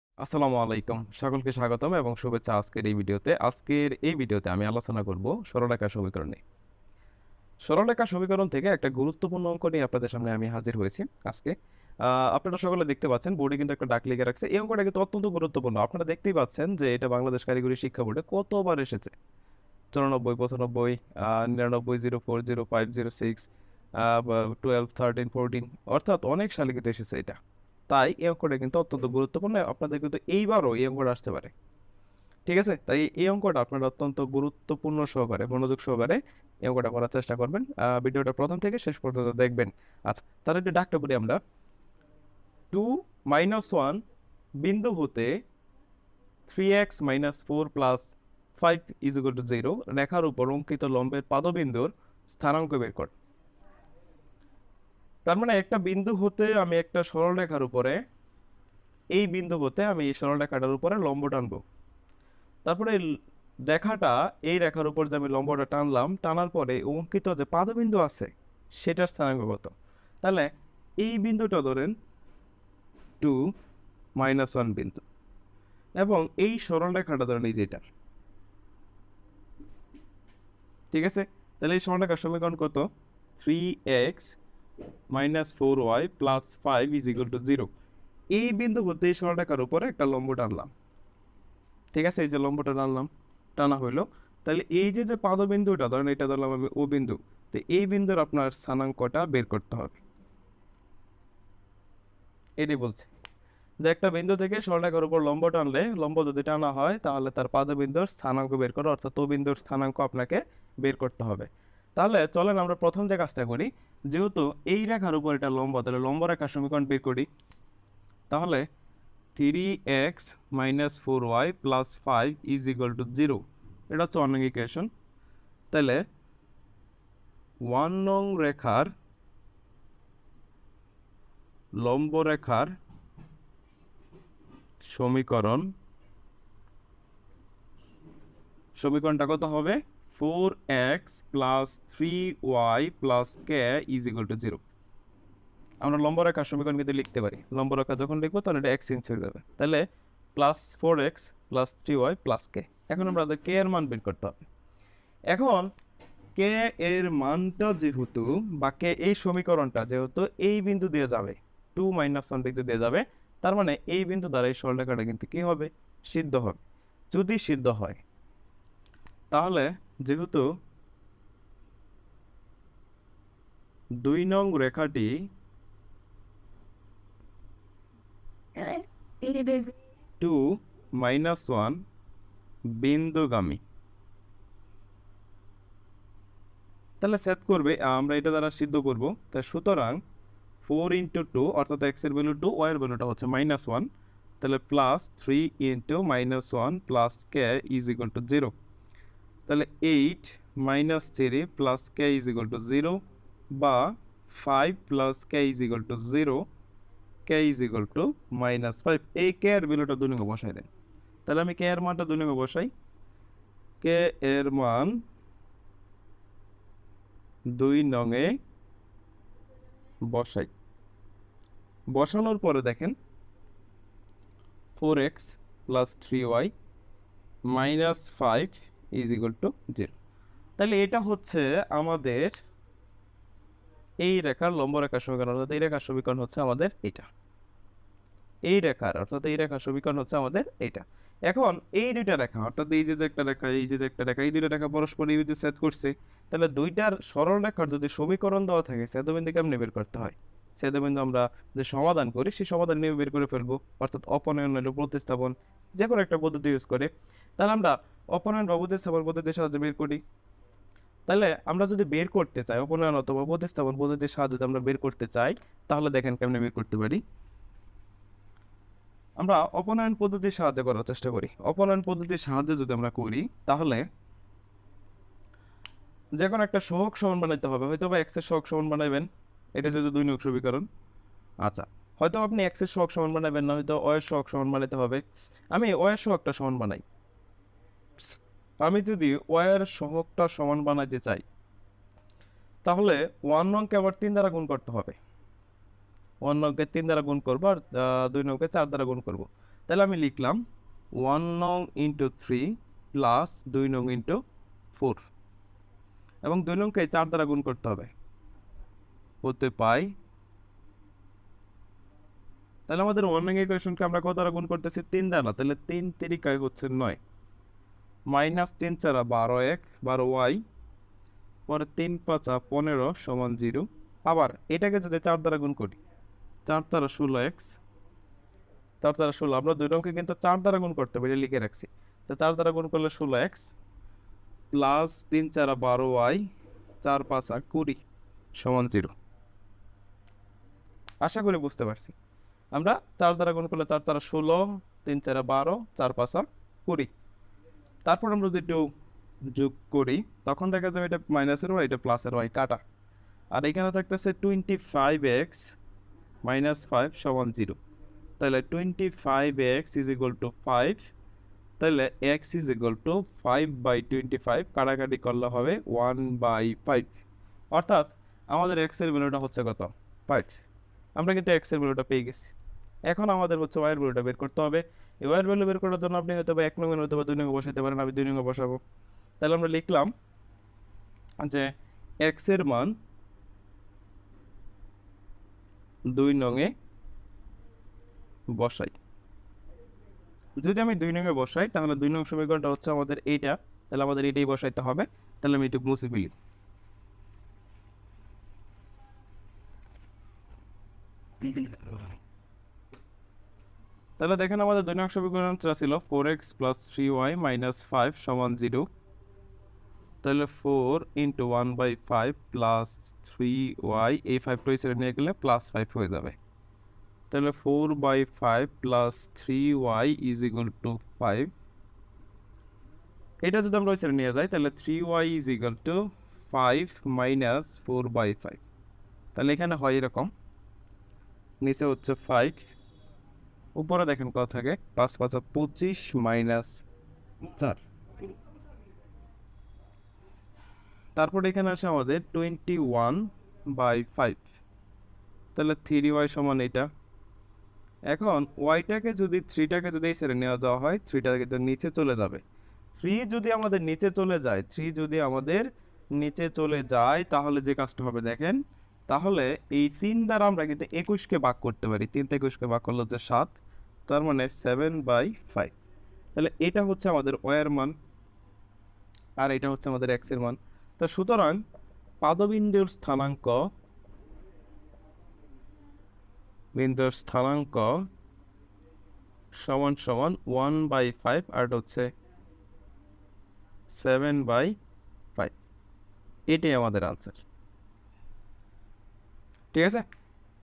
Diploma Mathematics 1। সরলরেখা সমীকরণ পাদ বিন্দুর স্থানাঙ্ক নির্ণয়। সুপার সাজেশন সমাধান ক্লাস।
📌আমাদের অনলাইন প্রাইভেট ক্লাস ও সুপার সাজেশন সংক্রান্ত তথ্য জানতে কল করুন 👉০১৭২৩-৪৭৪৪৪২